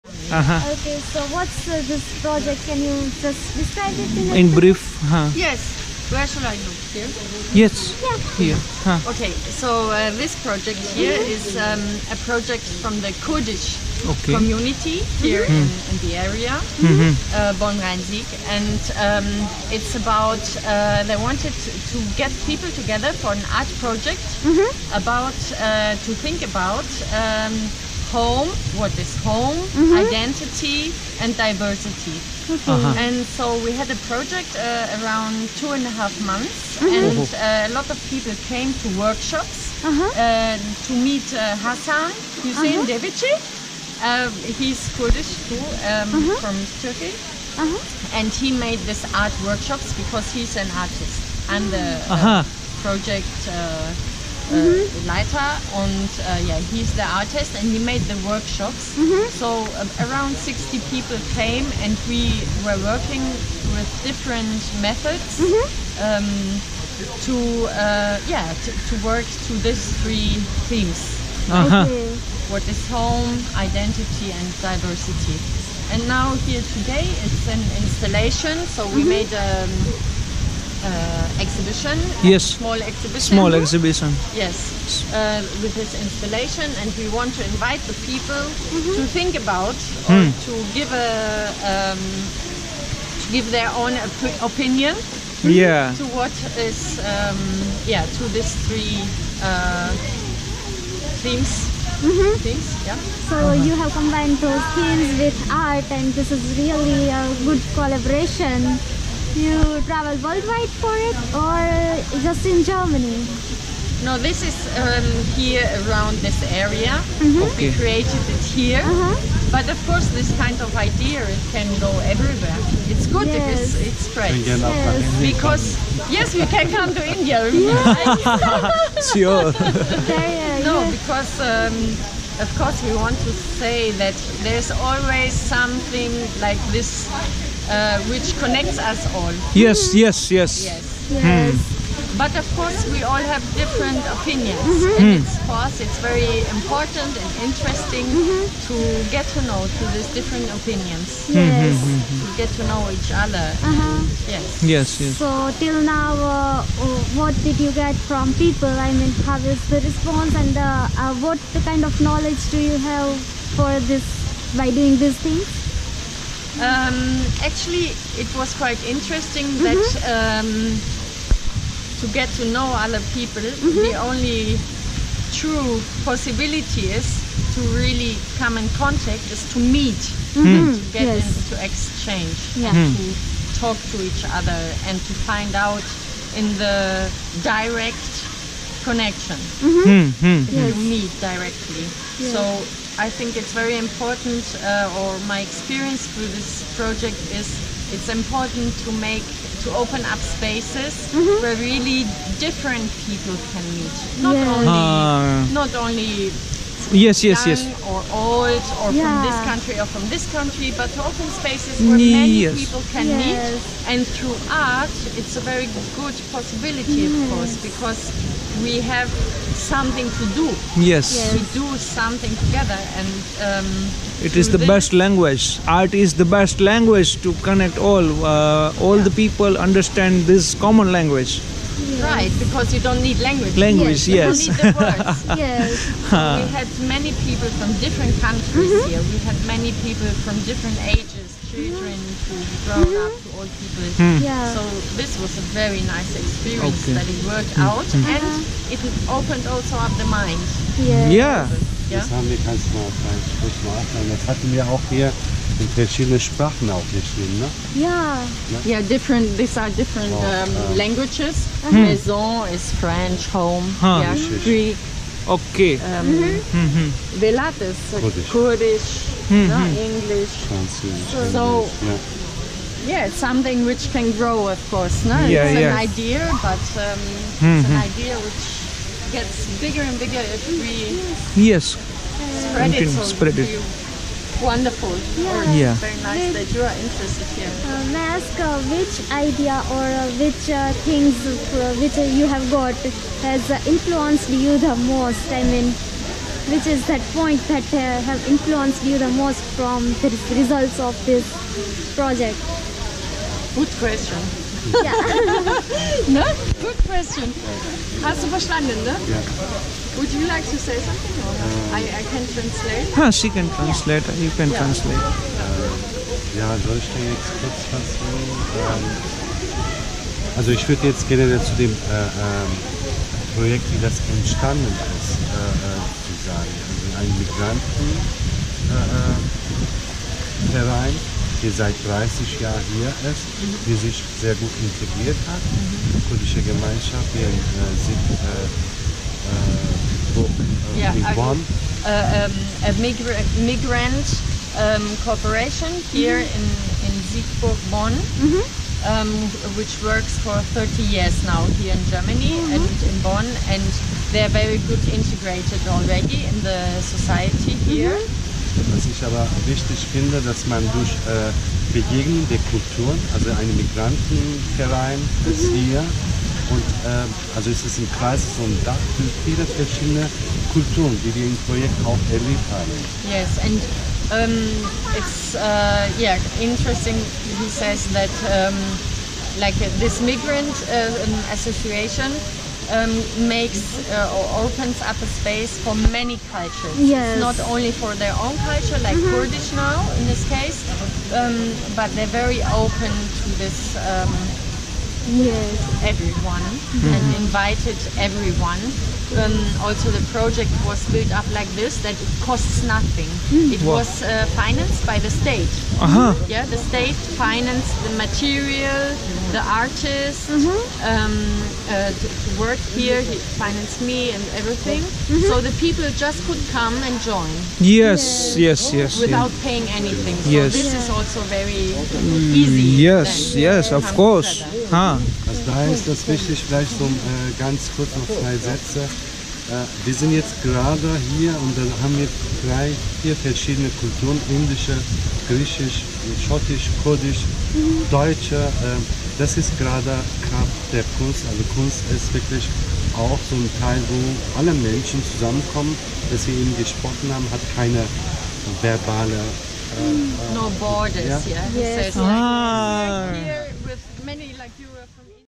emerging thought-collections of home, world and Identity | Artists' interview at Bonn, Germany
There are so many festivals in Germany. Carnivals, Oktoberfest, Cultural Festivals, Beer festival, Halloween Festival, Holi Festival, Dance Festival (Tanzfest), Music Festivals, Food festivals.. you mane it. We explored one such International Festival in Bonn near Köln. There were people from So many Countries. Here is how we accidentally bumped into one such wonderful experience while exploring Bonn, Germany and met people from Different countries.